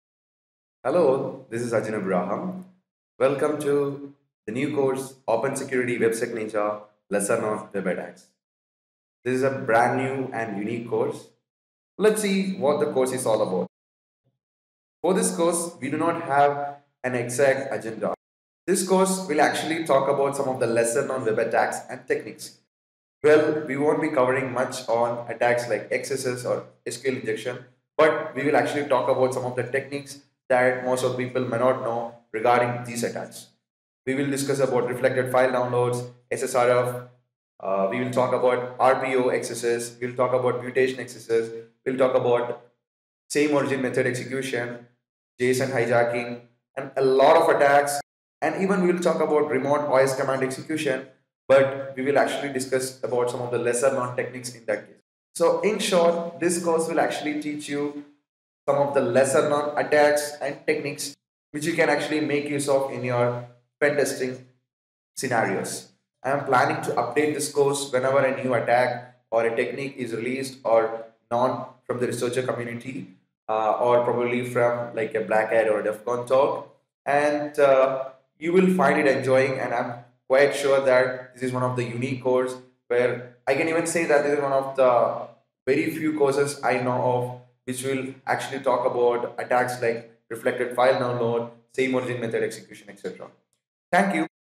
OpSecX WebSecNinja : The Lesser Known WebAttacks
WebSecNinja: Lesser Known WebAttacks is a brand new and unique web security course that takes the learner to the next level of web security.

Are you interested in exploring some of the exotic domains of web application security?
Want to learn about latest attack vectors in web security like Same Origin Method Execution (SOME) and Reflected File Download (RFD)?
Master the lesser known variants of XSS like Relative Path Overwrite XSS (rPO XSS) and Mutation XSS (mXSS)?

This course is for you!

A perfect blend of latest and lesser known web attacks that are explained with ultimate details and accompanied by demos and "how tos" that you can apply in real world red-team pentesting and security assessments. The course curriculum is designed to include web attacks and techniques that are not much documented in books, trainings, courses and elsewhere. This course doesn't have a fixed agenda and the purpose of that is to accommodate latest attacks as and when they are discovered or published on various conferences like BlackHat, DEFCON etc. Some of the attacks/ vulnerabilities explained in the course includes Same Origin Method Execution (SOME) or Reverse Clickjacking, Reflected File Download (RFD), lesser known techniques of Remote Command Execution (RCE) like detecting & exploiting blind RCE, bypassing weak RCE filters/WAF, Mutation XSS (mXSS), Relative Path Overwrite XSS (rPO XSS), Server Side Includes (SSI), Abusing Window's Opener property, JSON Hijacking, Server Side Request Forgery (SSRF) etc.

This course is best suited for web security architects, students, web developers, front-end javascript developers, security engineers, and security consultants. The contents of the course are well structured to satisfy all kind of audience from beginner to advanced level of competence. Most of the courses elsewhere focus on OWASP Top 10 web vulnerabilities and there exist a lot of attacks or vulnerabilities that are not documented or categorized in the OWASP Top 10 web vulnerabilities list. This course try to cover such attacks and techniques and helps you to become the Jedi Master of Web Security.